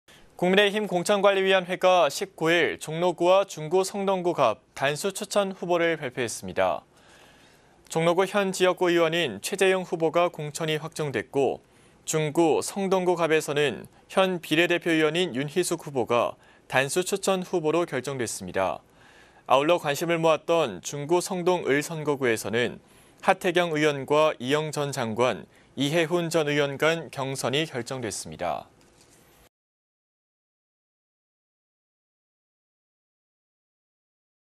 국민의힘, 종로 최재형, 중구성동갑 윤희숙 단수 공천 / 딜라이브TV
진행 : 박세종 아나운서

【 앵커멘트 】
국민의힘 공천관리위원회가
19일 종로구와 중구성동구갑 단수
추천 후보를 발표했습니다.

【 VCR 】
종로구 현 지역구 의원인
최재형 후보가 공천이 확정됐고
중구성동구갑에서는 현 비례
대표 의원인 윤희숙 후보가
단수 추천 후보로 결정됐습니다.

아울러 관심을 모았던 중구
성동을 선거구에서는 하태경
의원과 이영 전 장관, 이혜훈
전 의원 간 경선이 결정됐습니다.